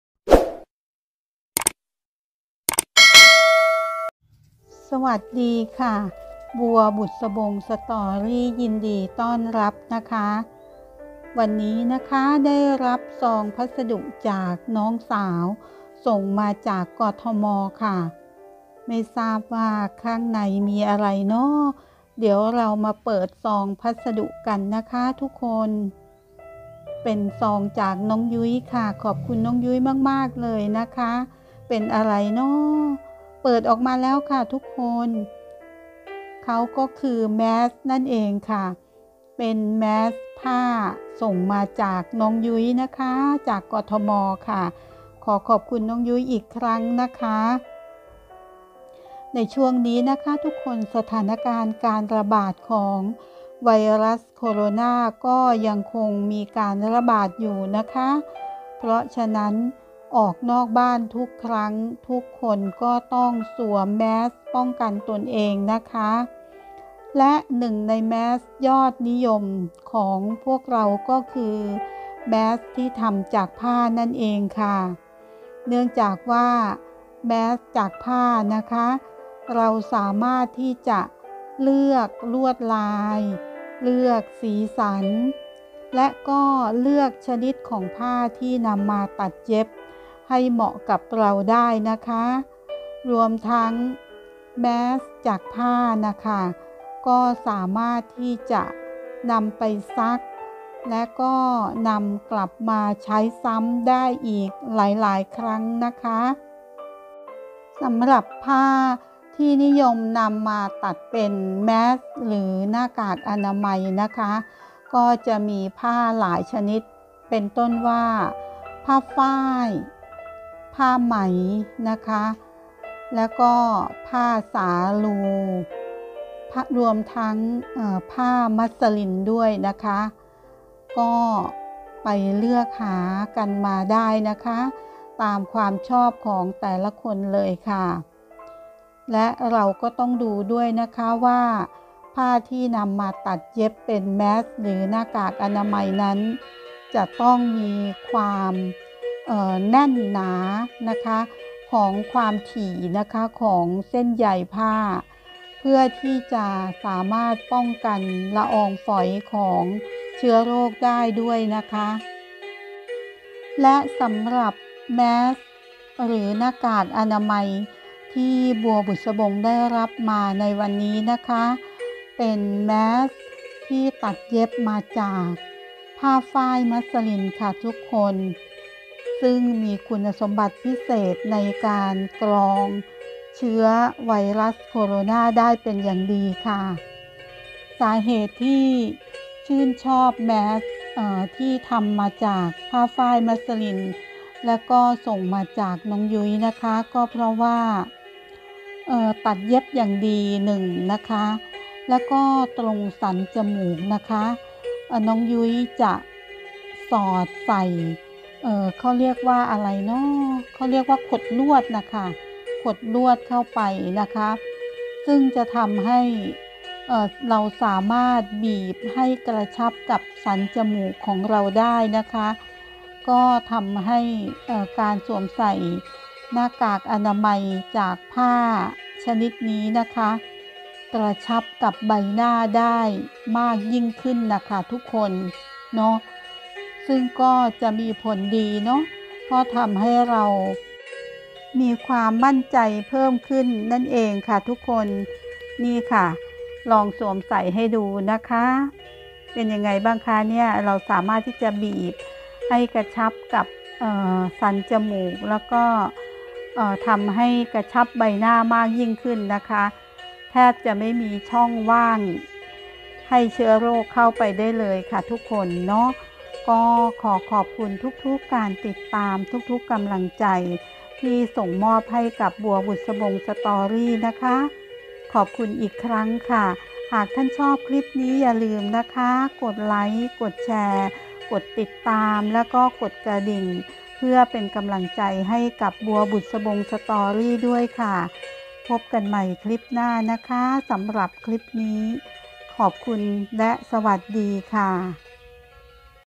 47/65 เปิดซองพัสดุ
เปิดซองพัสดุ ข้างในคือแมสผ้าที่ตัดเย็บจากผ้าฝ้ายมัสลิน มีขดลวดตรงสันจมูกเพื่อเพิ่มความกระชับในการสวมให้แนบชิดใบหน้ามากยิ่งขึ้น#แมสผ้าฝ้าย#หน้ากากอนามัยจากผ้าฝ้าย#บัวบุษบง story